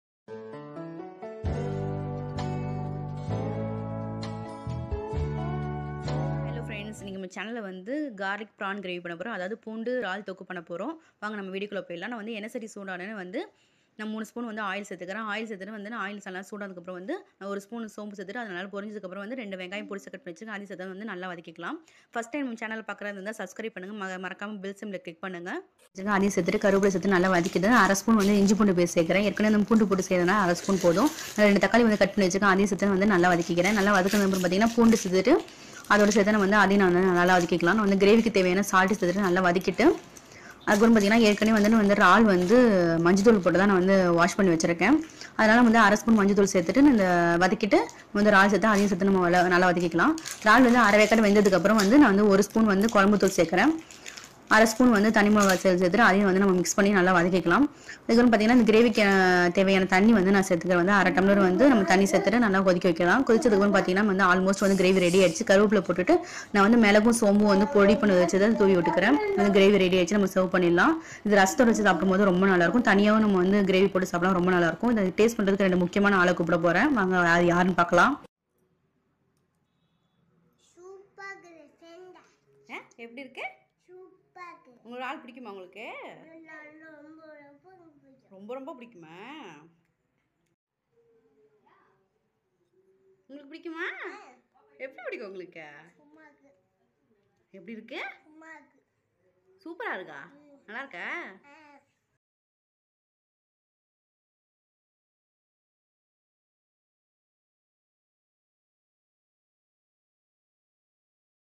Garlic prawn🍤🍤🍤 gravy| Easy and tasty 😋😋😋 make it fast recipe
garlic prawn gravy side dish for rasam, sambar. easy and tasty gravy. so simple less ingrediants. make it fast and easy recipe. kavinish shorts# make it fast and easy recipe. must try it.